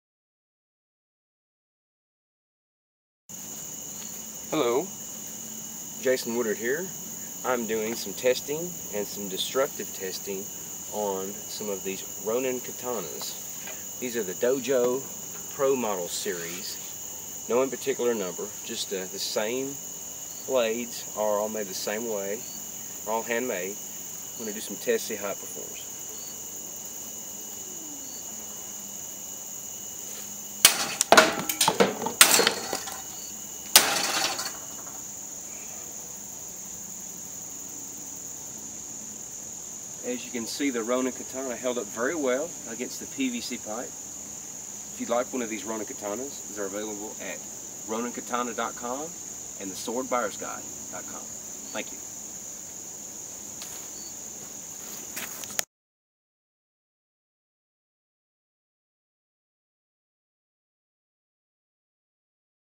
Ronin Katana Dojo Pro Samurai Sword Cutting & Destruction Test #6
presents: Sword designer Jason Woodard volunteered to make a series of cutting tests for Ronin. During this series he will cut soft targets like bottles, all the way up to complete destruction on metal and concrete.